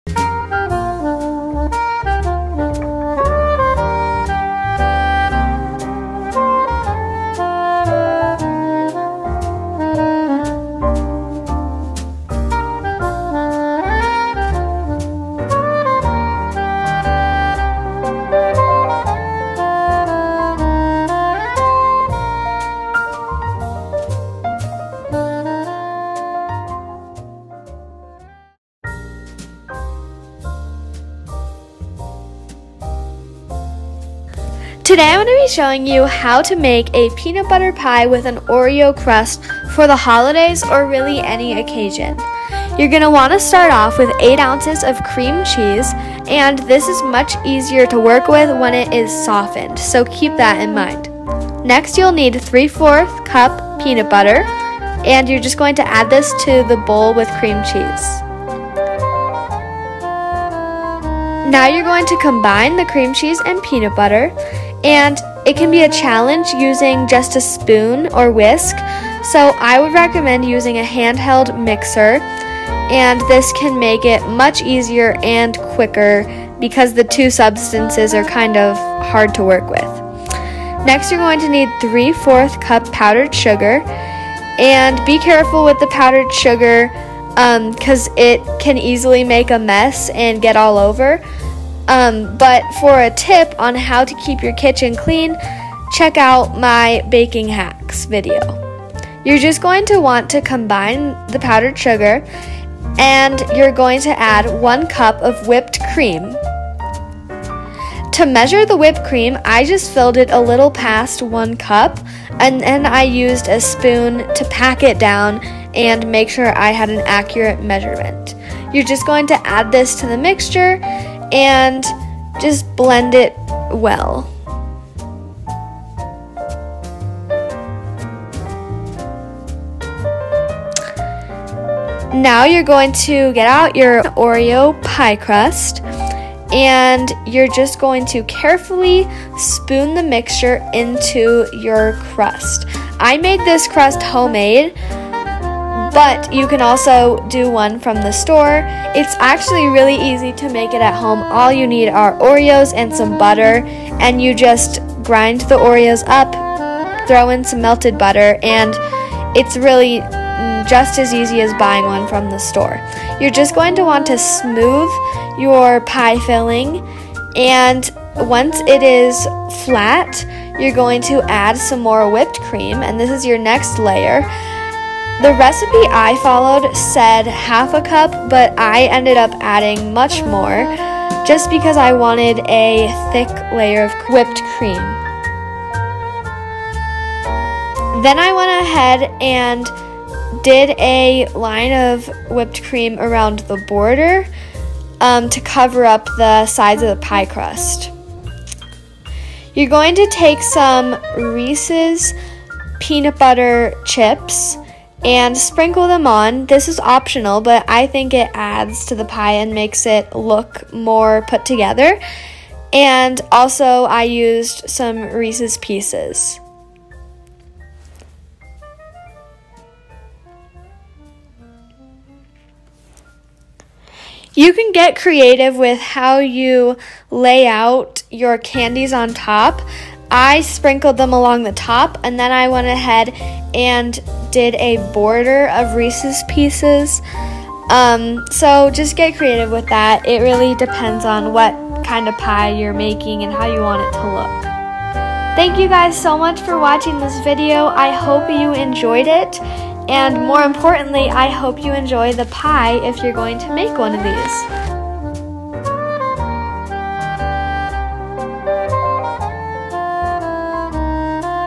Peanut Butter Pie
In this video you will learn how to make peanut butter pie. This is perfect for the holidays or really any time of year! :)